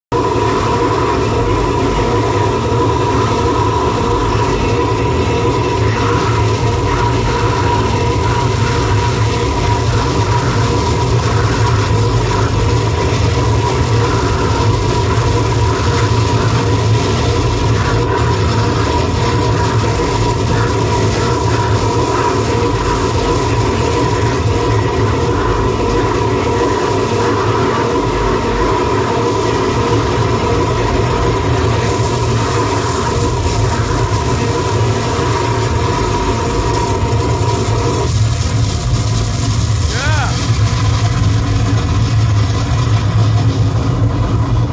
ACTUARY LIVE @ SOUP 4/30/16
A CLIP OF A LIVE PERFORMANCE IN OCHIAI JAPAN 2016